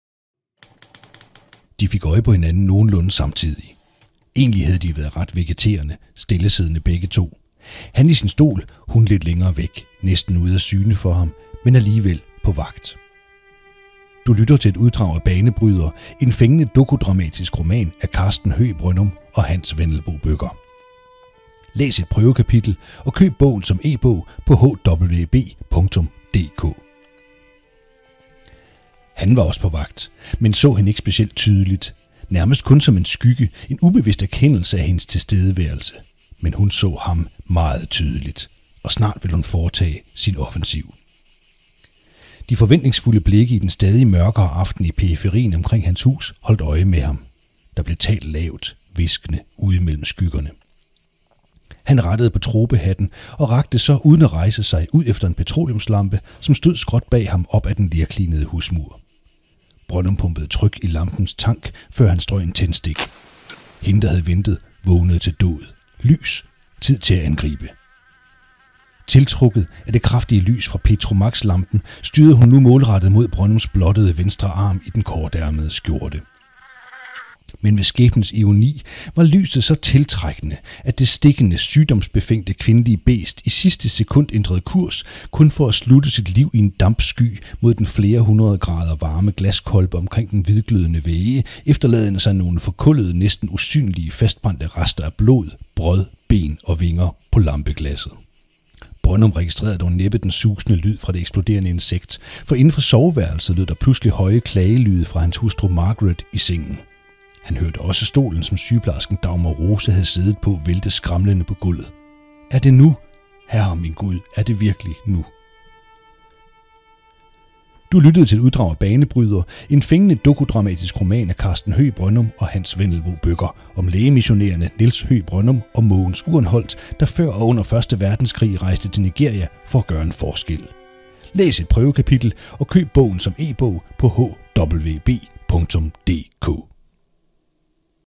BANEBRYDER - e-bog
BANEBRYDER
Dokudramatisk roman af Karsten Brønnum og Hans Wendelboe Bøcher om to ganske ualmindelige danskeres dramatiske liv og skæbne i Nigeria før og under 1. verdenskrig.

BANEBRYDER er en biografisk fortælling om to ganske usædvanlige mænds ganske usædvanlige liv; en beretning om to fascinerende personligheder med på én gang fælles og meget forskellige skæbner. Samtidig er det fortællingen om de omstændigheder og personer, som på forunderlig og dramatisk vis på afgørende måde påvirkede deres liv.

Bogen bygger på historiske data og er gennemresearchet i danske og udenlandske arkiver og gennem flere rejser til Nigeria.

Bogen har forord af Henrik Engelbrekt Refshauge, generalsekretær i Mission Afrika og er rigt illustreret med originale fotos fra Mission Afrikas arkiver.

BANEBRYDER findes endnu kun som dansk e-bog.

218 sider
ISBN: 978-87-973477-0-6
180 kr.